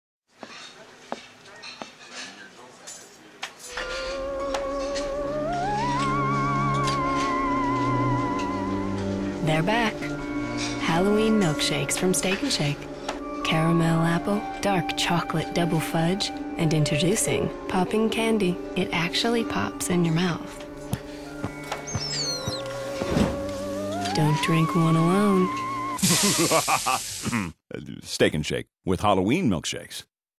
Shake Reflection
Steak n Shake TV ad for their Halloween shakes, including one with candy that pops in your mouth.